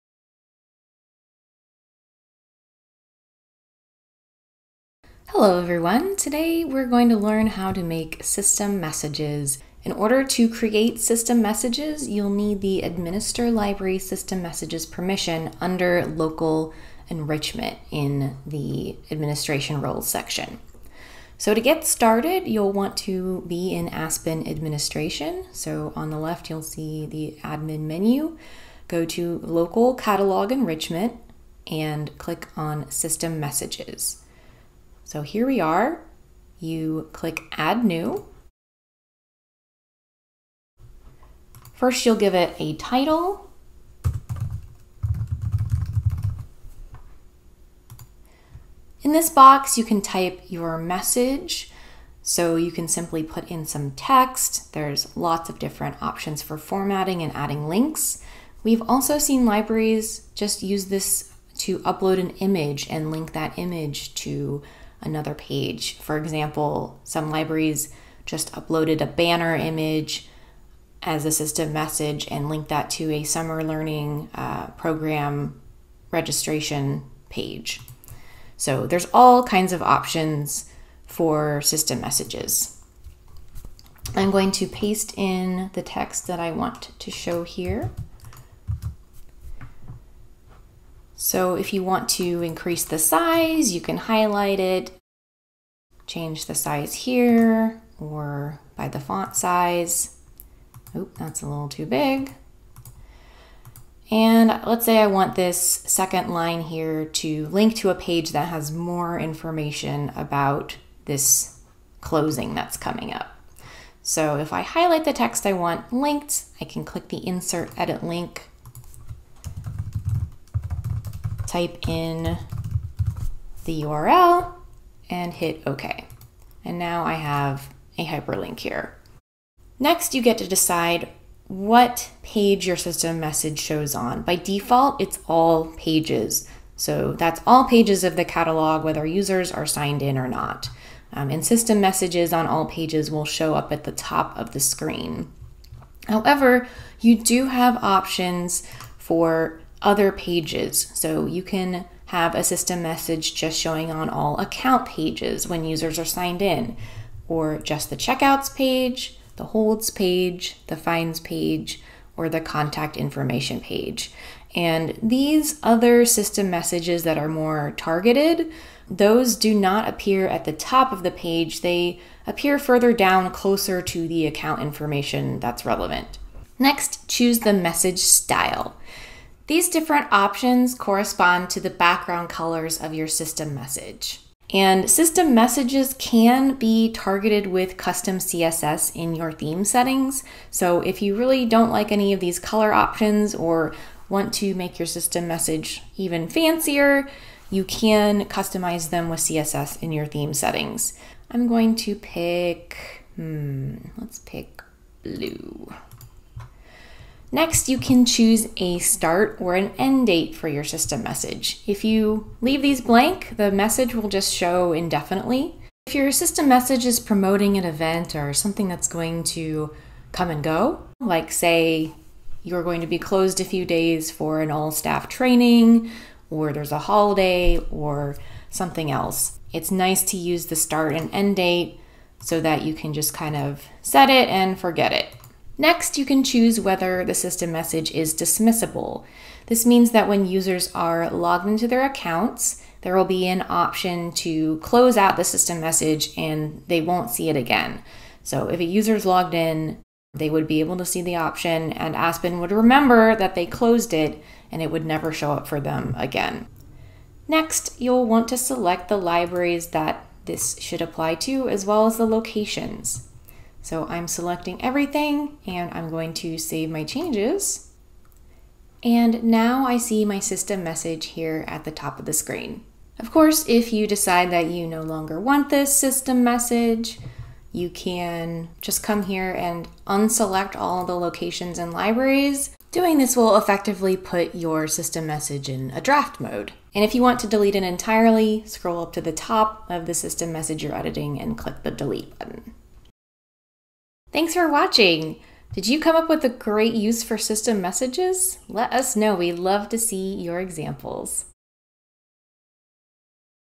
System Messages
Learn how to create system messages in Aspen Discovery.